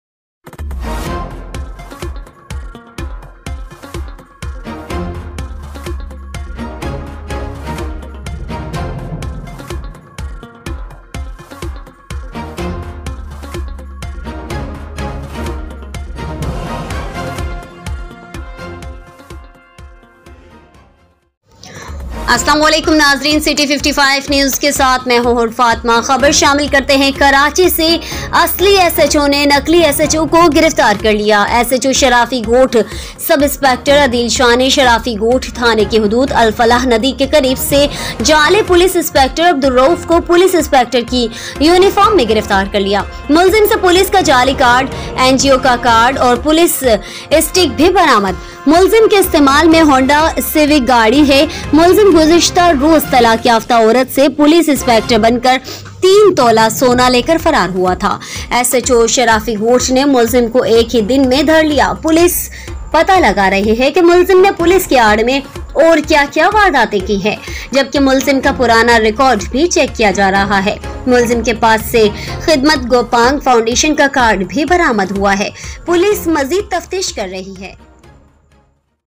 کراچی اصلی ایس ایچ او نے نقلی ایس ایچ او کو گرفتار کرلیا،
کراچی: ایس ایچ او شرافی گوٹھ سب انسپکٹر عدیل شاہ نے شرافی گوٹھ تھانے کی حدود الفلاح ندی کے قریب سے جعلی پولیس انسپکٹر عبدالرؤف کو پولیس انسپکٹر کی یونیفارم میں گرفتارکرلیا،
ملزم سے پولیس کا جعلی کارڈ ، این جی او کا کارڈ  اور پولیس اسٹک بھی برآمد ملزم کے استعمال میں ہونڈا سوک گاڑی ہےملزم گزشتہ روز طلاق یافتہ عورت سے پولیس انسپکٹر بن کر  تین تولہ سونا لے کر فرار ہوا تھا،ایس ایچ او شرافی گوٹھ نے ملزم کو ایک ہی دن میں دھر لیا
پولیس پتہ لگارہی ہے کہ ملزم نے پولیس کی آڑ میں اور کیا کیا وارداتیں کی ہیں، جبکہ ملزم کا پرانا ریکارڈ بھی چیک کیا جارہا ہے
ملزم کے پاس  سےخدمت گوپانگ فاونڈیشن کا کارڈ بھی برآمد ہوا ہے ، پولیس مزید تفتیش کررہی ہے